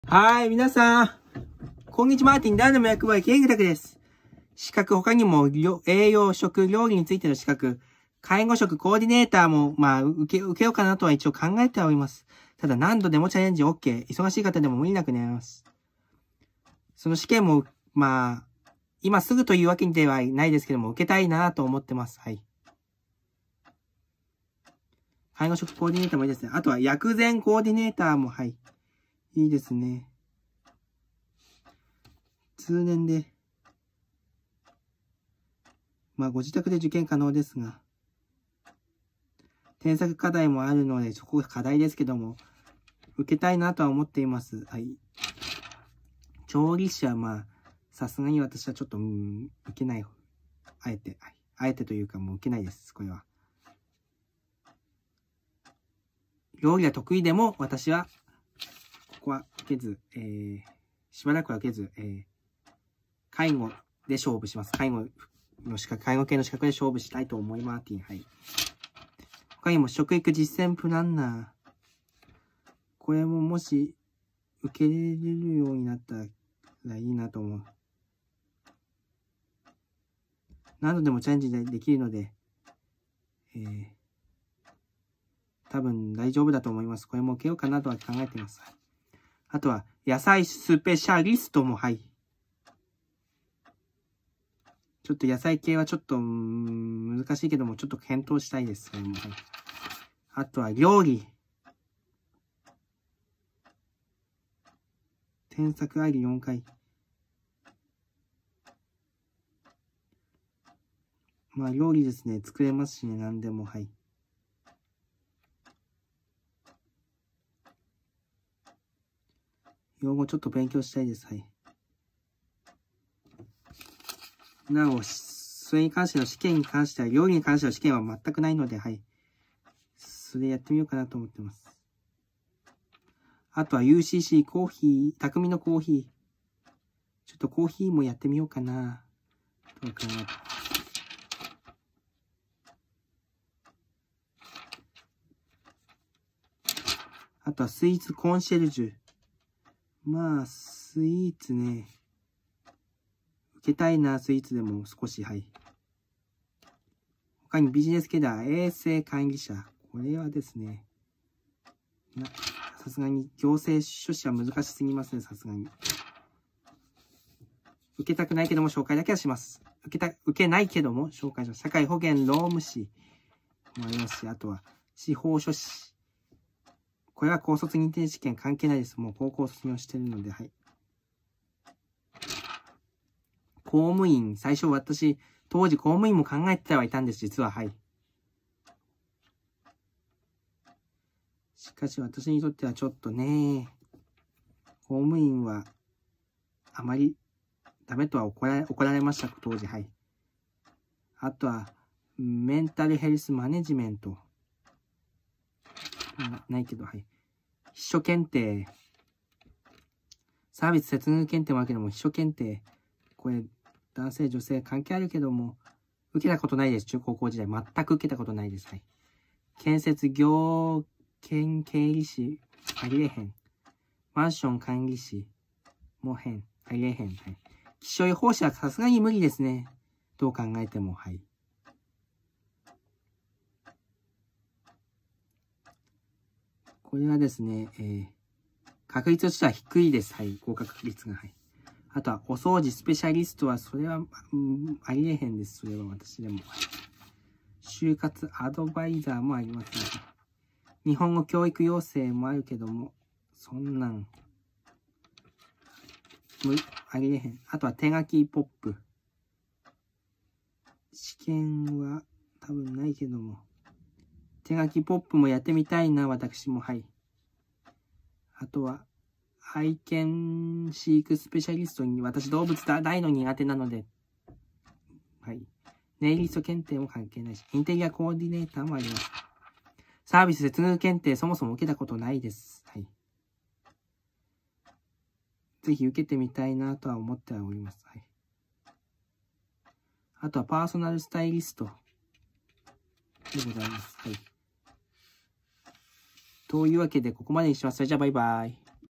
キングたくのYJ#732
資格パート3。料理、食、栄養、サービス、ビジネスなどの資格について。その中でもぜひ挑戦してみたいものや、自分はどう考えてもだめな資格あるが一応紹介。